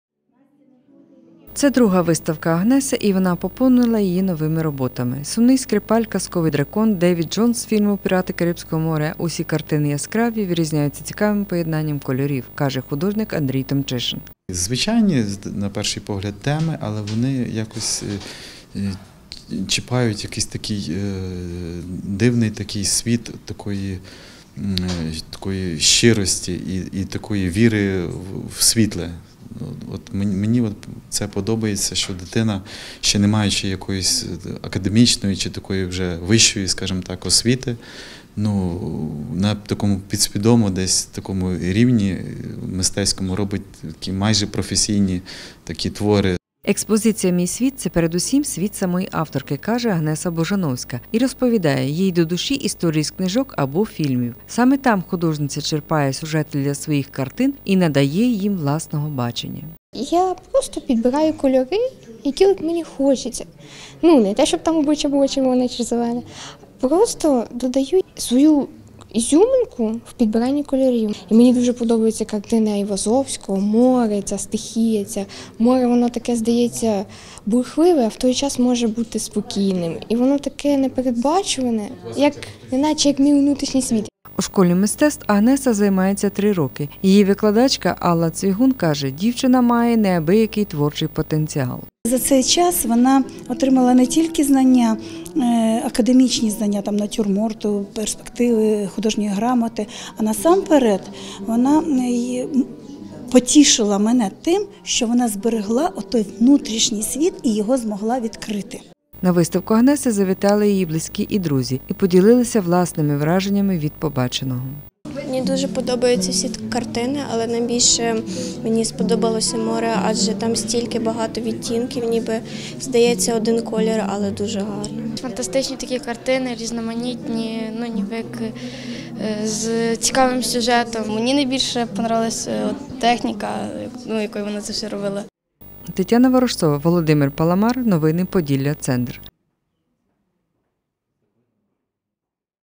Експозиція картин «Мій світ» Агнеси Бужановської запрошує у світ казки
“Мій світ” - експозиція із такою назвою відкрилась у мистецькій вітальні Хмельницької бібліотеки для дітей імені Шевченка. Свої художні роботи представила учениця дитячої школи мистецтв Агнеса Бужановська. Побувала там і наша кореспондентка Тетяна Ворожцова.